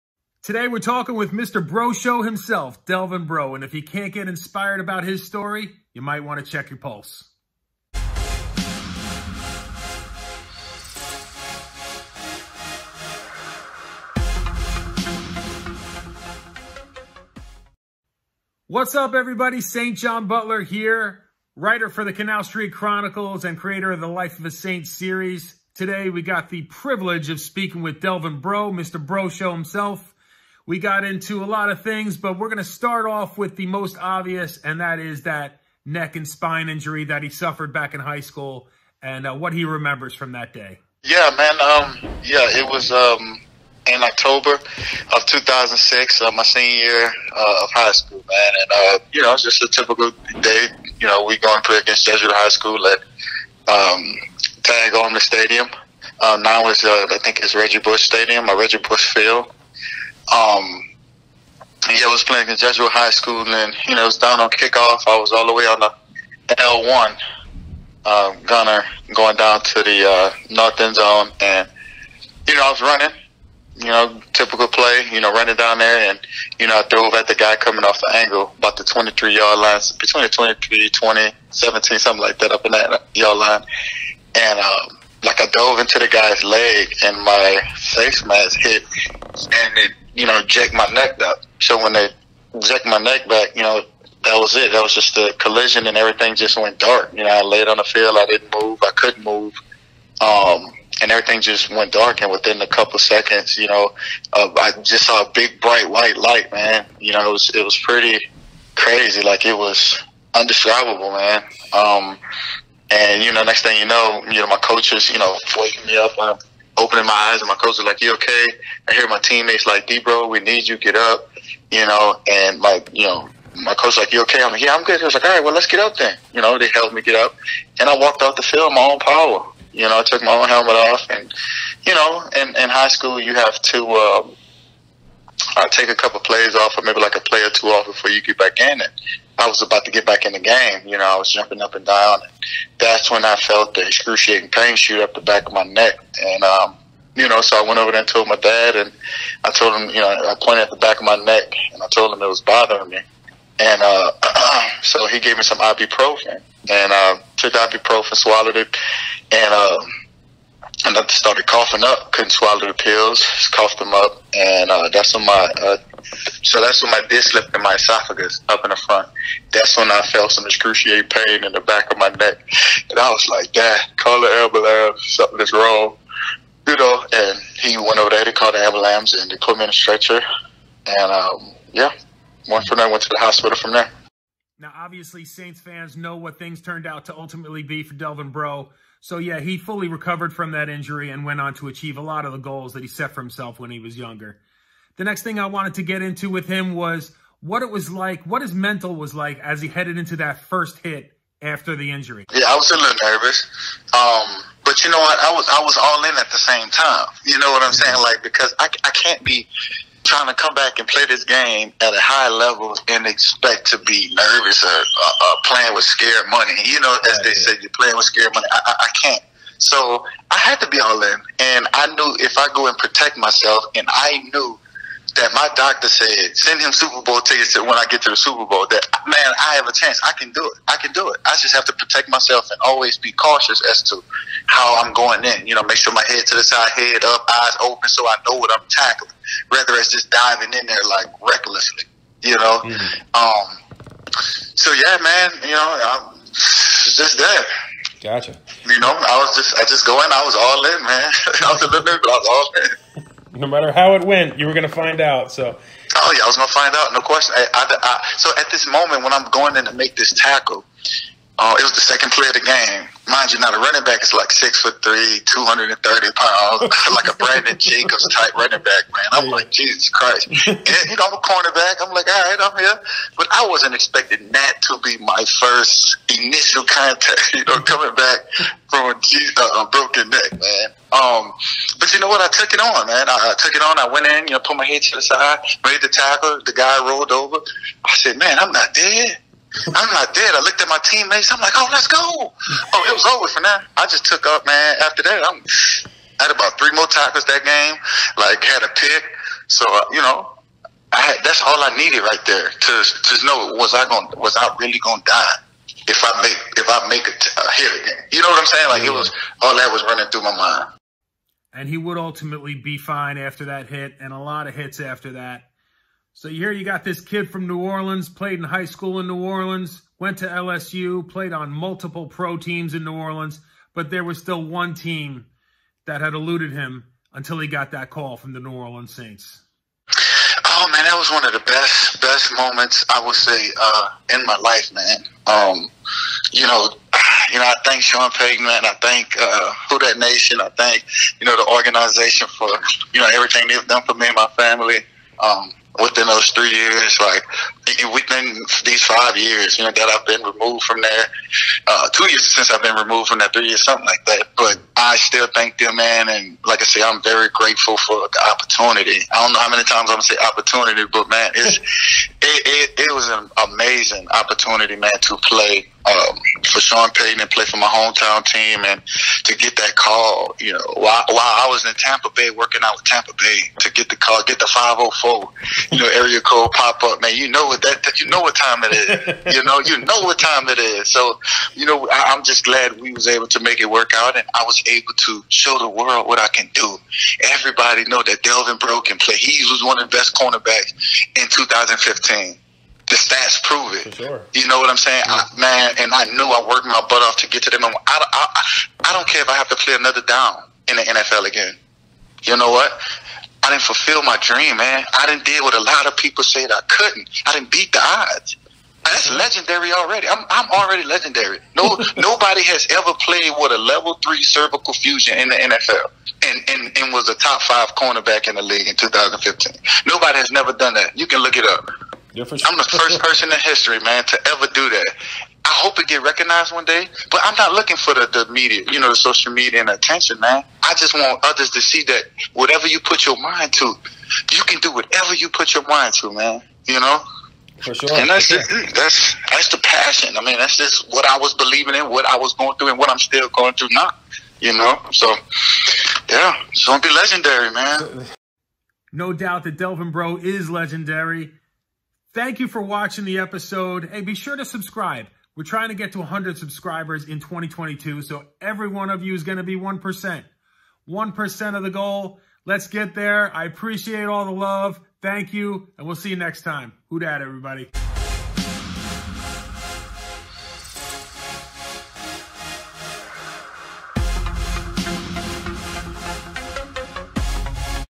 Delvin Breaux Talks Injury, Recovery and the New Orleans Saints
Delvin Breaux inspires with his message of persistence and determination. Breaux talks about his neck injury in high school, what it was like taking the field for the first time after that injury and when he got the call form the New Orleans Saints in Episode 3 of Life of a Saint.

You can follow me on both Twitter and Instagram: @saintjohnbutler

Chapters
00:00 Delvin Breaux Talks Injury, Recovery and the New Orleans Saints
00:40 Delvin Breaux describes what he remembers from the spine injury he suffered during a game in high school.
03:22 Delvin Breaux talks about what his mental state was like the first time he had to make a hit after his injury.
05:30 Delvin Breaux remembers when he got the call to play for the New Orleans Saints.